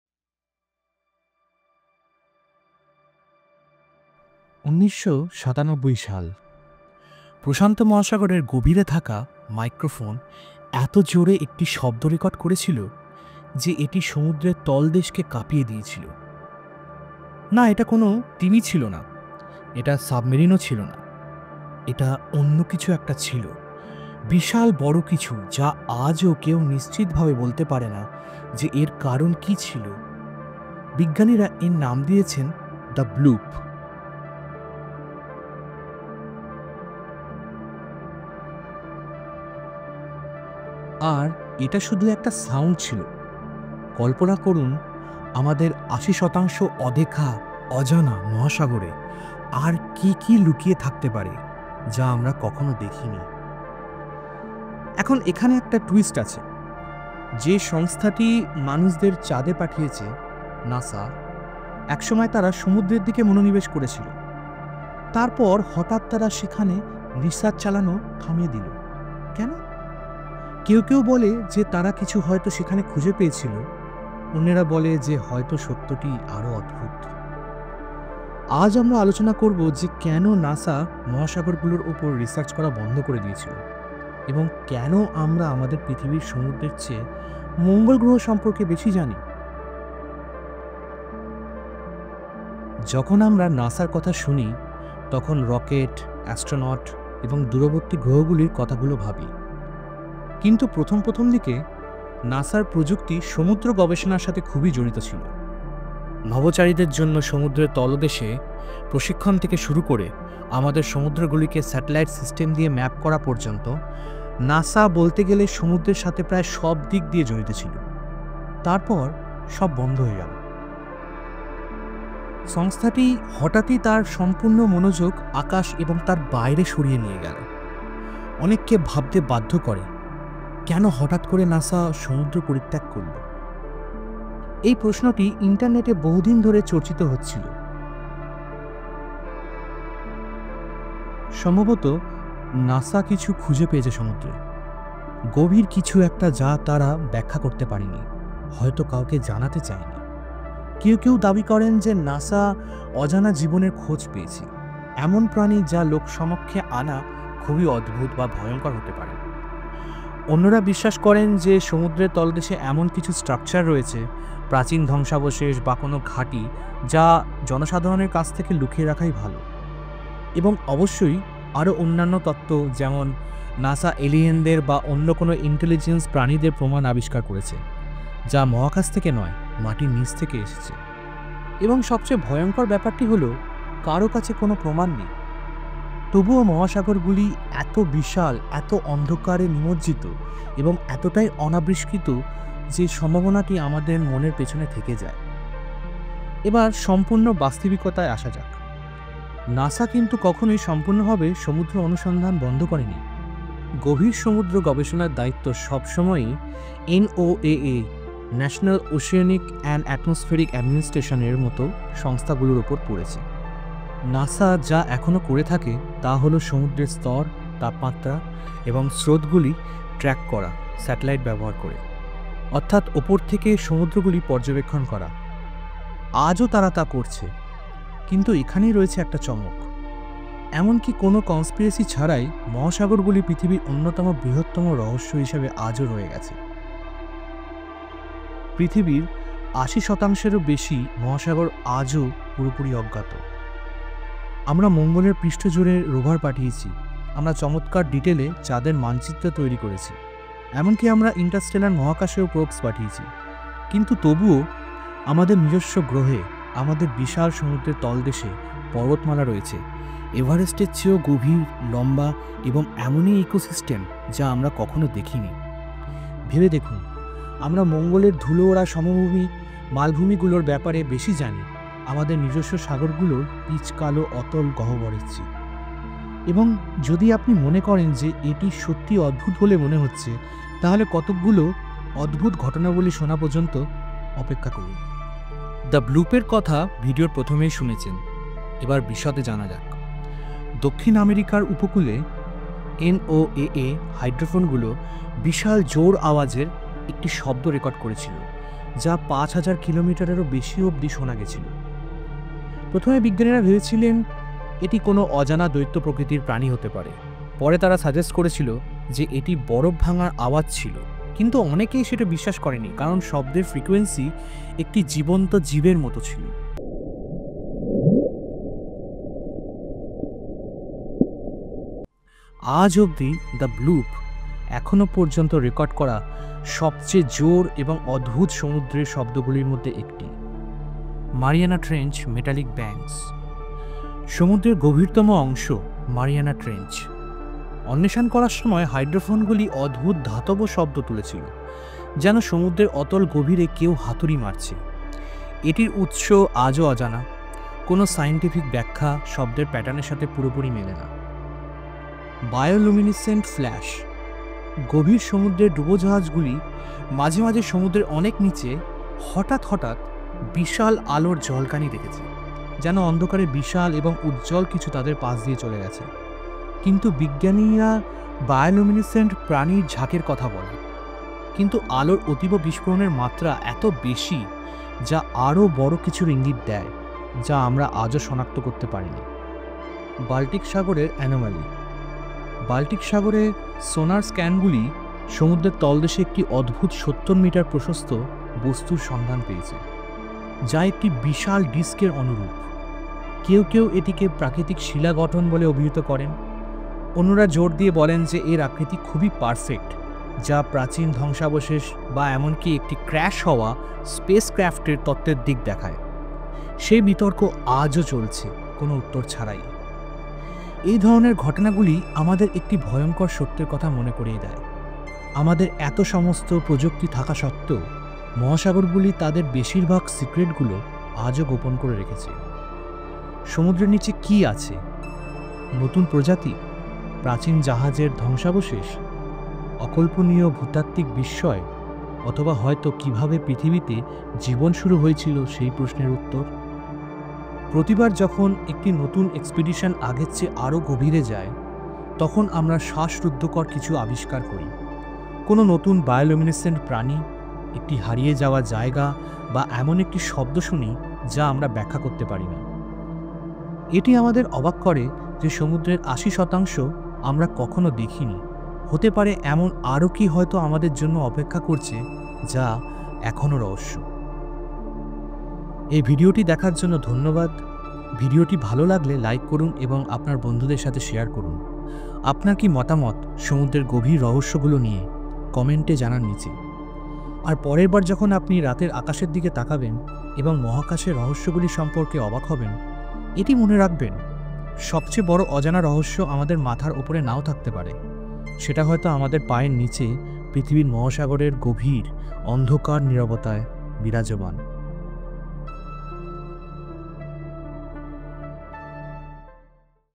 Why Did NASA Stop Exploring the Ocean? | The Bloop - এখনো রহস্য (Episode 14) | Bengali
In 1997, deep in the Pacific Ocean, an underwater microphone recorded a sound so loud it could be heard from thousands of kilometers away. Scientists called it The Bloop — but to this day, no one knows exactly what caused it.

And here’s the twist… NASA, the agency famous for sending humans to the Moon, once turned its focus to exploring Earth’s oceans. And then, suddenly stopped.

In this episode, we dive into:
🌊 The mysterious Bloop sound of 1997
🚀 Why NASA shifted away from ocean exploration
🌐 How 80% of our oceans remain uncharted
🔍 Why we know more about Mars than our own seas

Could the greatest mystery of our planet be hiding in the deep?

Image credit - various sources